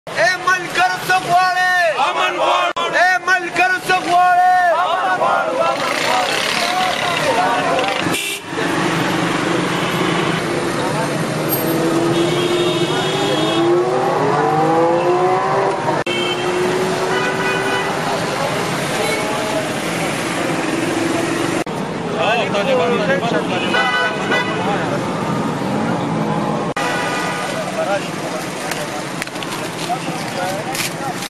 د باچاخان پوهنتون زده کوونکیو په یاد شوګیره
پر باچا خان پوهنتون د وسله وال برید خلاف چارسېدالو له مړو او ژوبلو زده کوونکیو سره د پیوستون په موخه شوګیره نیولې او د هغوی په یادې کې یې شمعې بلې کړې.